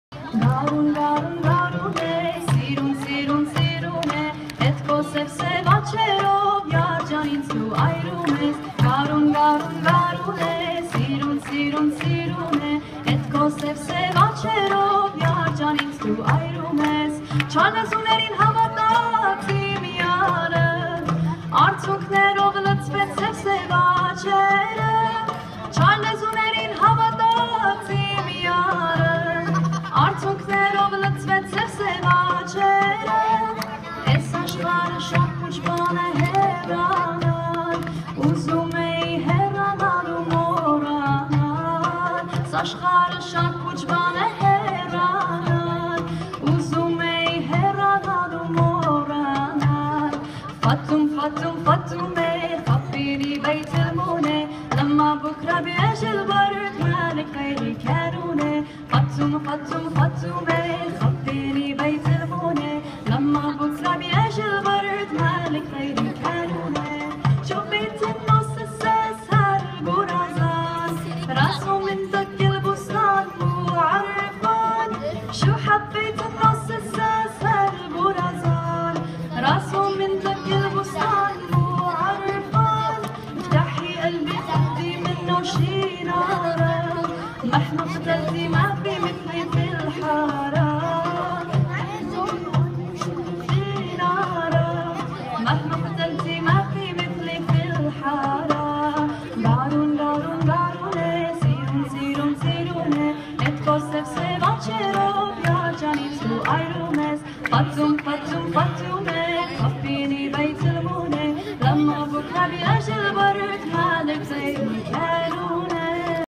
Armenian-Arabic Song 2
UC Irvine Armenian Student Association 4th Annual Cultural Festival. February 24, 2016